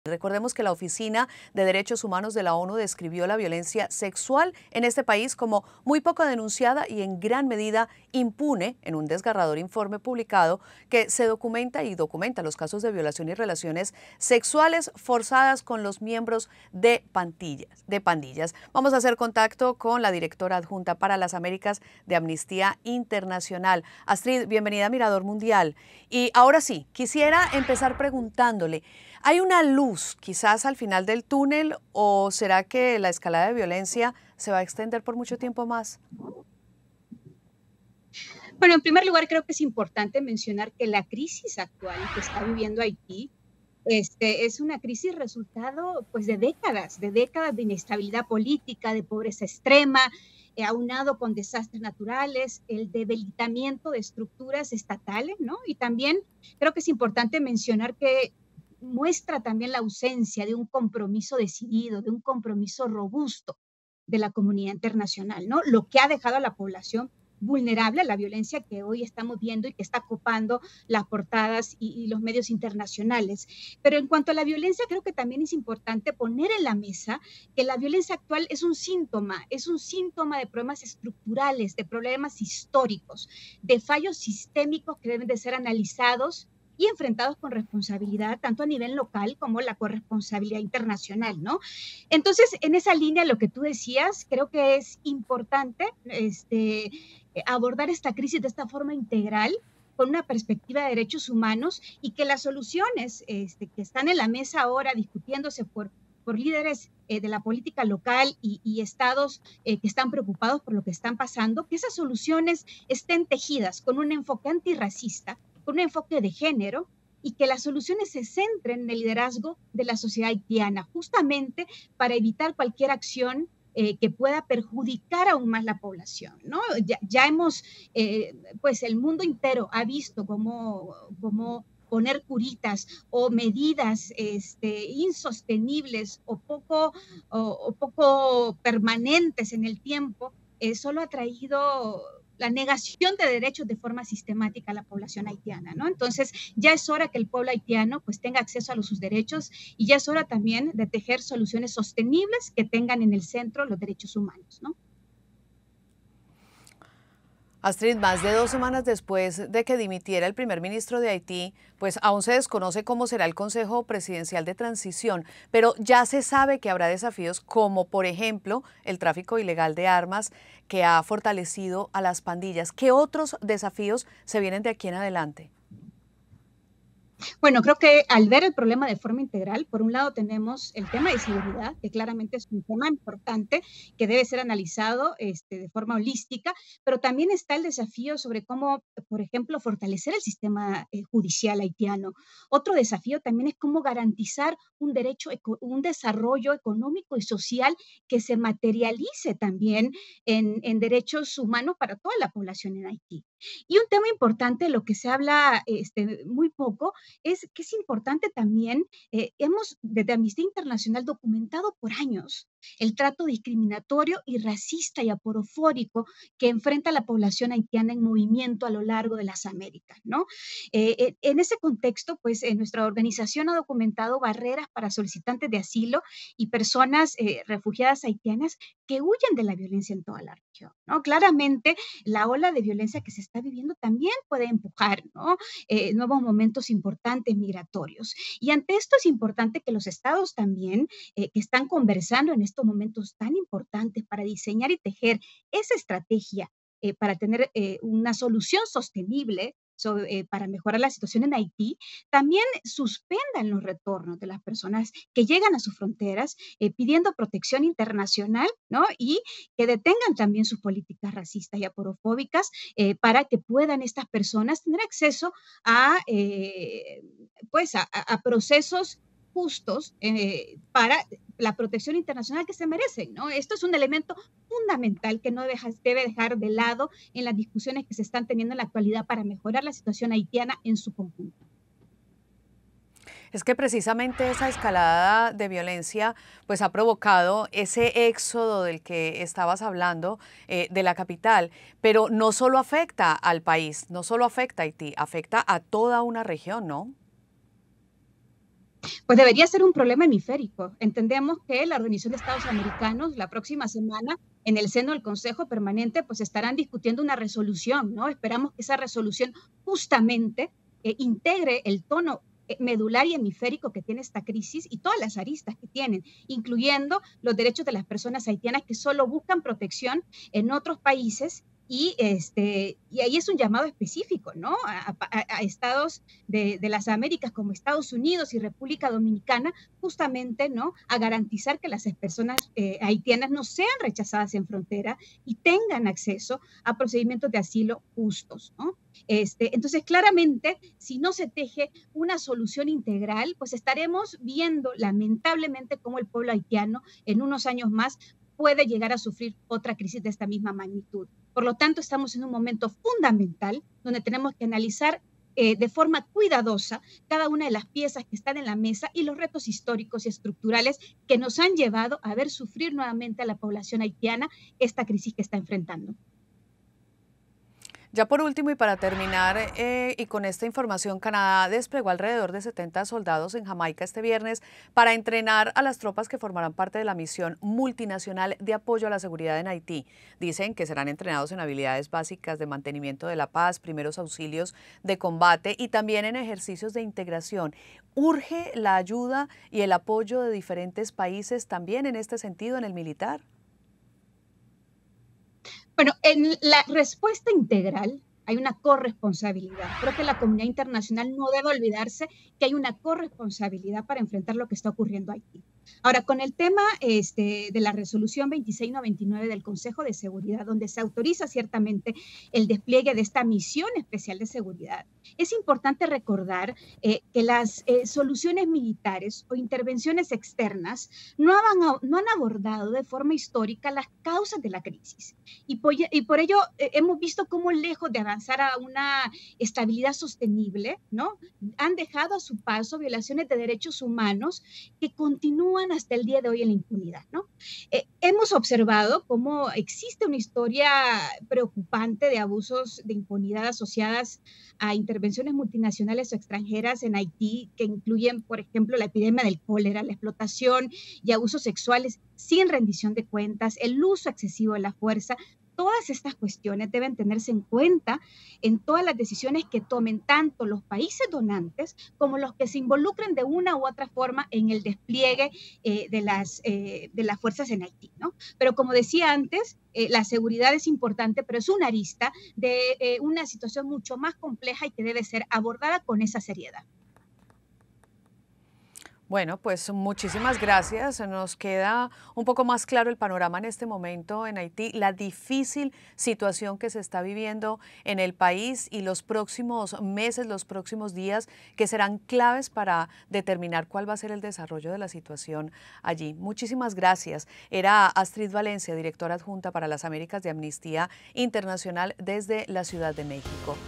Violencia en Haití es “un síntoma de problemas históricos", dice especialista
La violencia en Haití sigue agravándose y el panorama está lejos de ser alentador. ¿Cómo llegó el país a este presente? Lo responde Astrid Valencia, directora adjunta para las Américas de Amnistía Internacional.